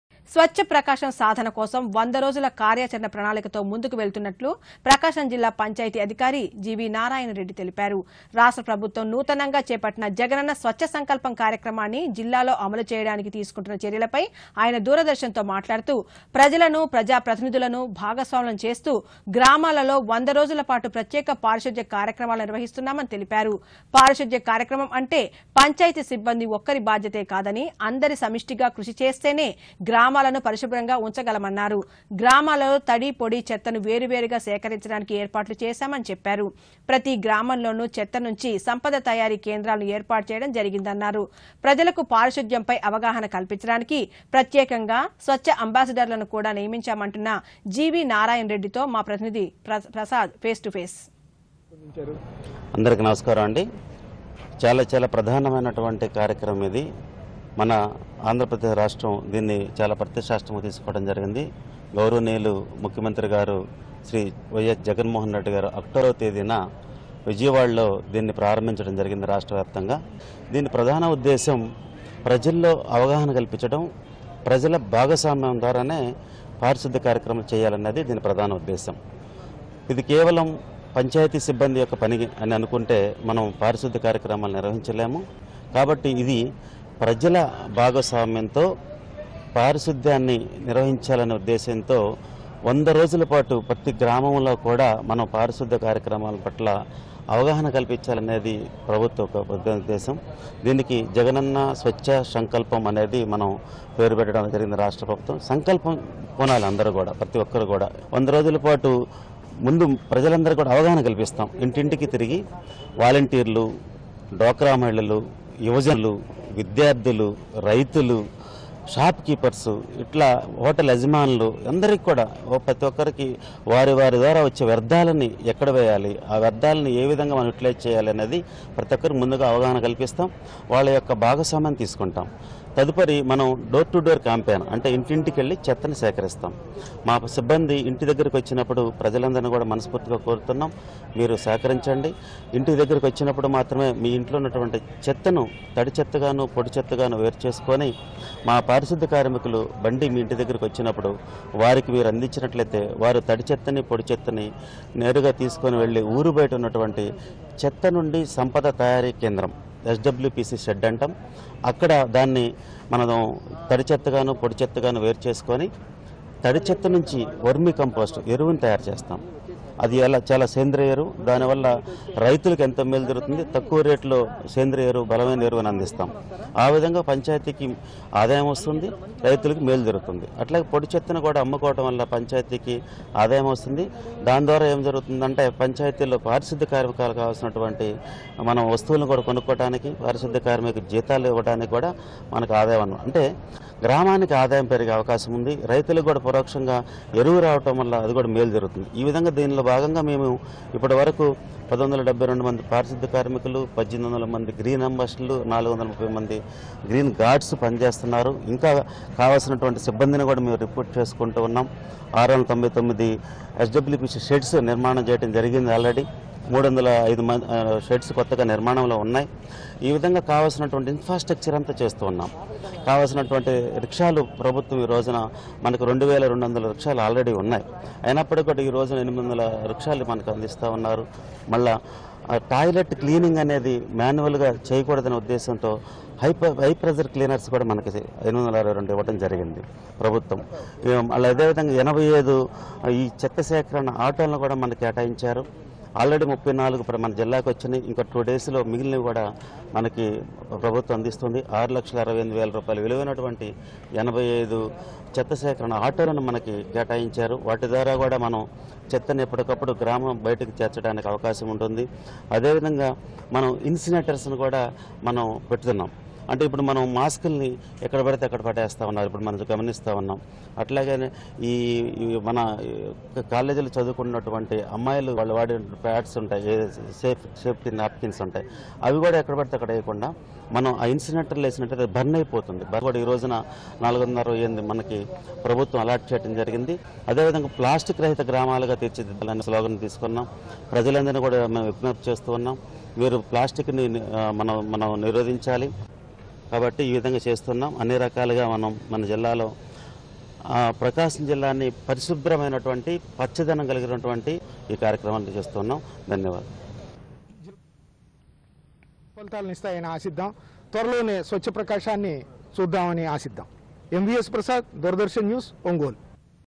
🔴స్వచ్ఛ ప్రకాశం కోసం కృషి చేస్తున్నామన్న జిల్లా పంచాయతీ అధికారి జి.వి.నారాయణ రెడ్డితో ఫేస్ టు ఫేస్
స్వచ్ఛ ప్రకాశం సాధన కోసం 100 రోజుల కార్యాచరణ ప్రణాళికతో ముందుకు వెళుతున్నట్టు ప్రకాశం జిల్లా పంచాయతీ అధికారి జి.వి.నారాయణ రెడ్డి అన్నారు. రాష్ట్ర ప్రభుత్వం నూతనంగా చేపట్టిన జగనన్న స్వచ్ఛ సంకల్పం కార్యక్రమాన్ని జిల్లాలో అమలు చేయడానికి తీసుకుంటున్న చర్యలపై ఆయన దూరదర్శన్ తో మాట్లాడుతూ, ప్రజలను, ప్రజాప్రతినిధులను భాగస్వాములను చేస్తూ గ్రామాలలో 100 రోజుల పాటు ప్రత్యేక పారిశుద్ద్య కార్యక్రమాలను నిర్వహిస్తున్నామన్నారు. పారిశుద్ద్య కార్యక్రమం అంటే పంచాయతీ సిబ్బంది ఒక్కరి బాధ్యతే కాదని... అందరూ సమష్టిగా కృషి చేస్తేనే గ్రామాలను పరిశుభ్రంగా ఉంచగలమన్నారు. గ్రామాలలో తడి,పొడి చెత్తను వేరువేరుగా సేకరించడానికి ఏర్పాట్లు చేశామని చెప్పారు. ప్రతి గ్రామంలోనూ చెత్త నుంచి సంపద తయారీ కేంద్రాలను ఏర్పాటు చేయడం జరిగిందన్నారు. ప్రజలకు పారిశుద్ద్యంపై అవగాహన కల్పించడానికి ప్రత్యకంగా స్వచ్ఛ అంబాసిడర్లను కూడా నియమించామంటున్న జి.వి.నారాయణరెడ్డితో మాప్రతినిధి ప్రసాద్ ఫేస్ టు ఫేస్.